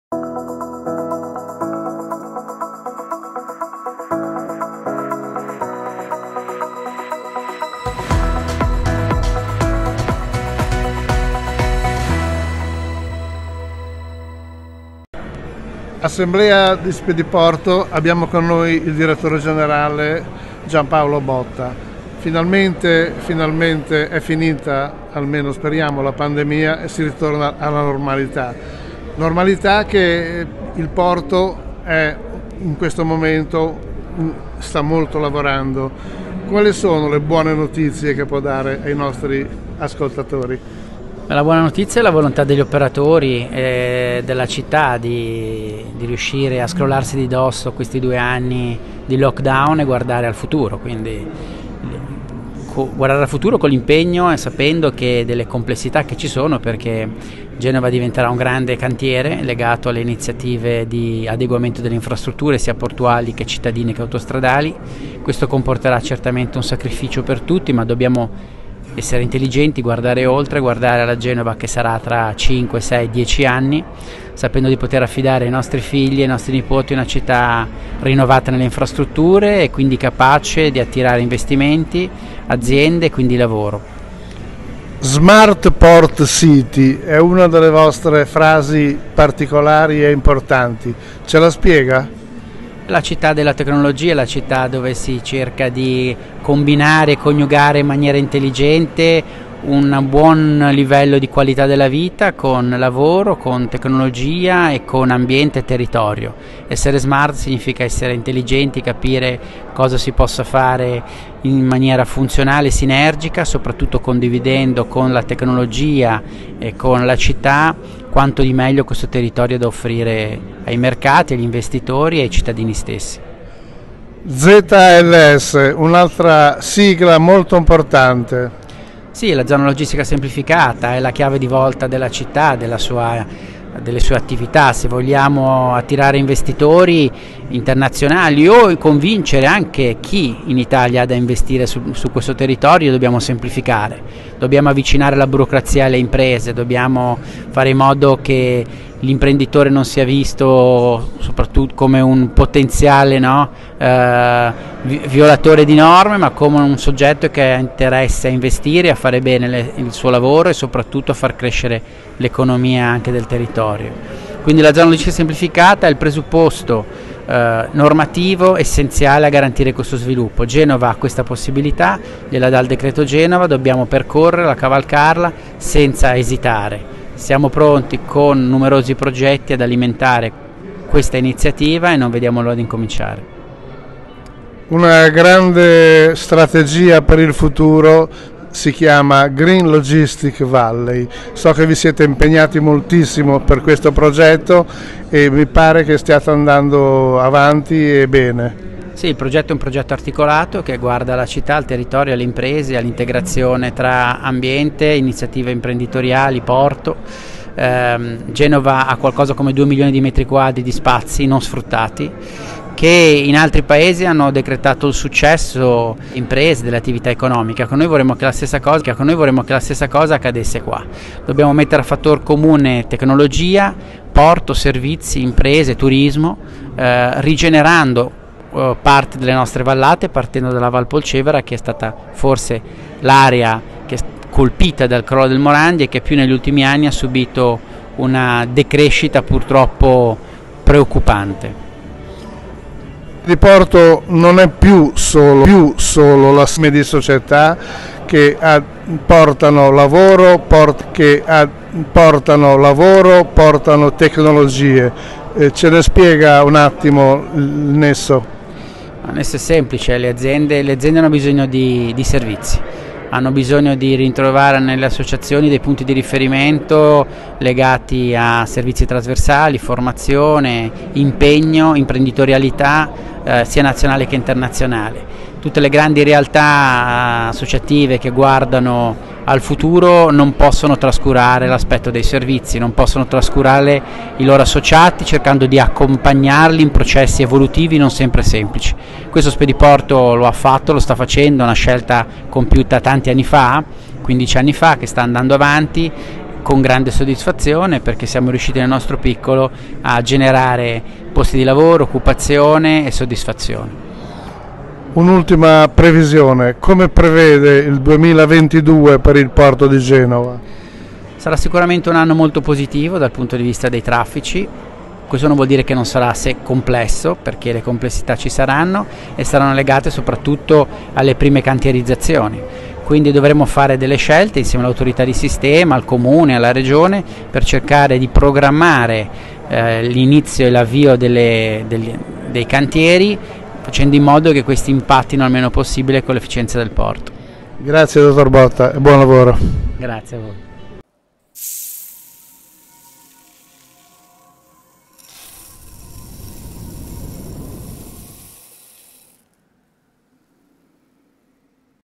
Spediporto | Assemblea Pubblica 16.05.22 | Intervista a Giampaolo Botta
Genova - Intervista a Giampaolo Botta, direttore Generale Spediporto, durante l'Assemblea Pubblica del 16 Maggio 2022

Fonte: Liguria Today